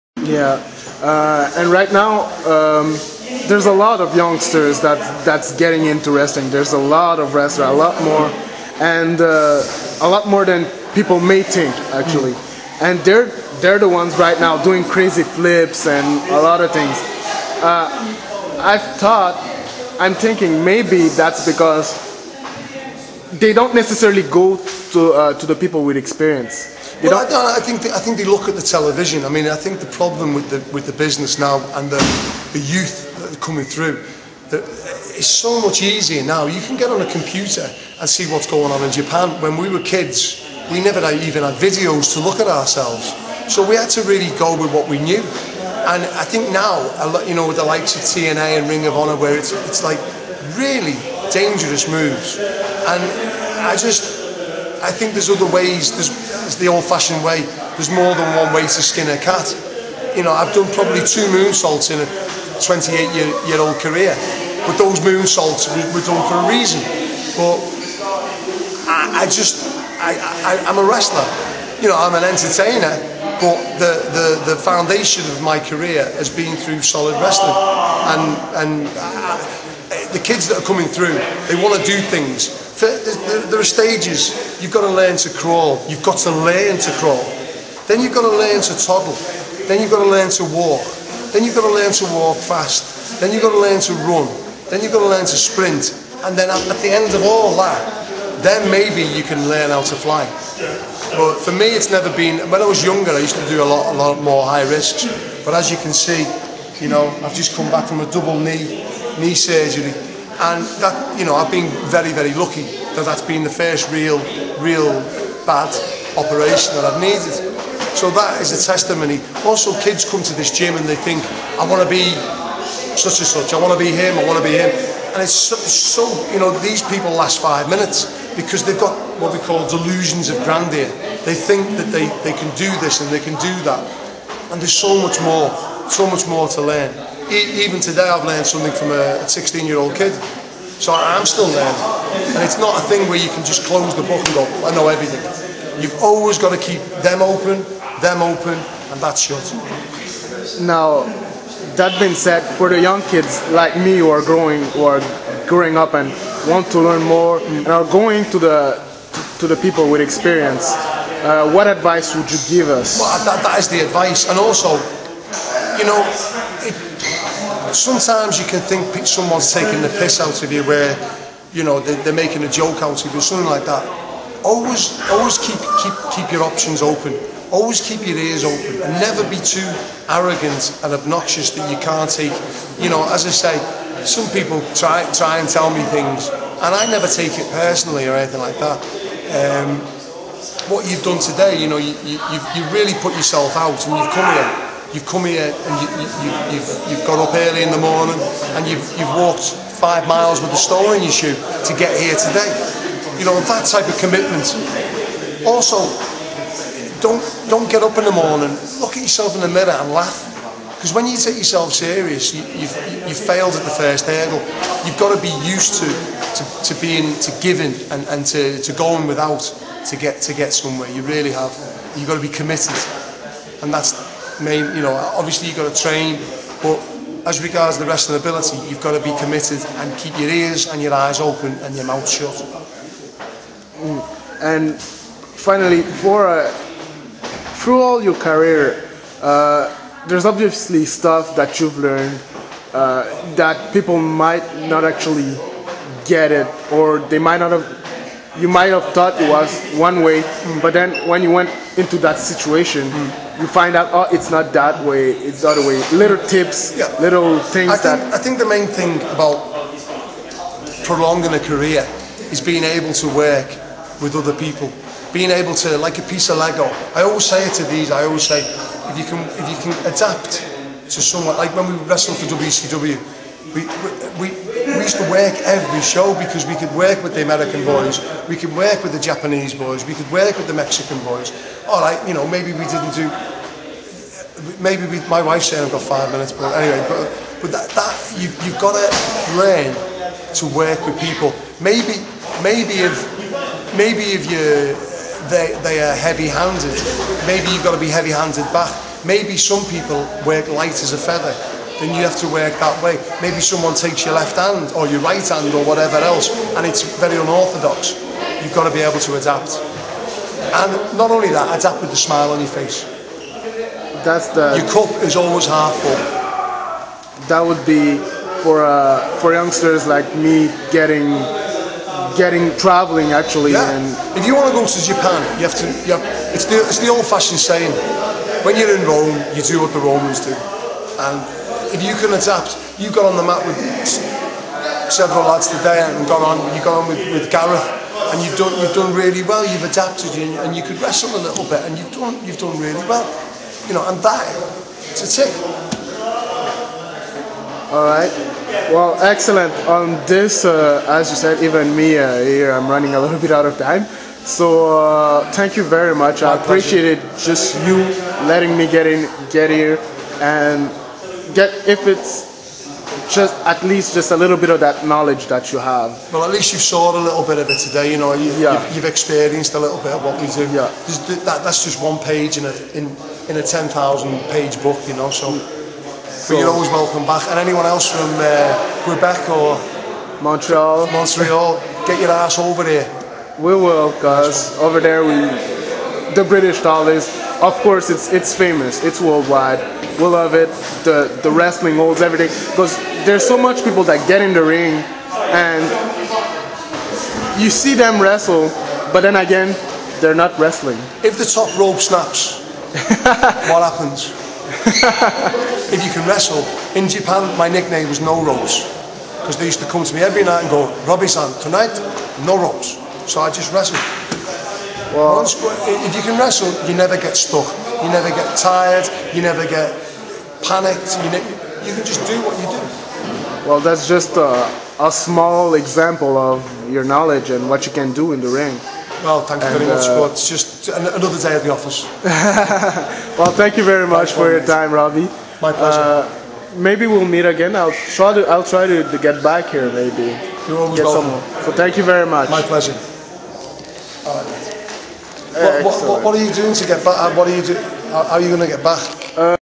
Seth Seifer's Trip To England: Interview with THE U.K Legend Robbie Brookside ! Part 2
After the training seminar, Robbie Brookside takes some time apart to answer a couple of questions and give us a insight on what wrestling really is.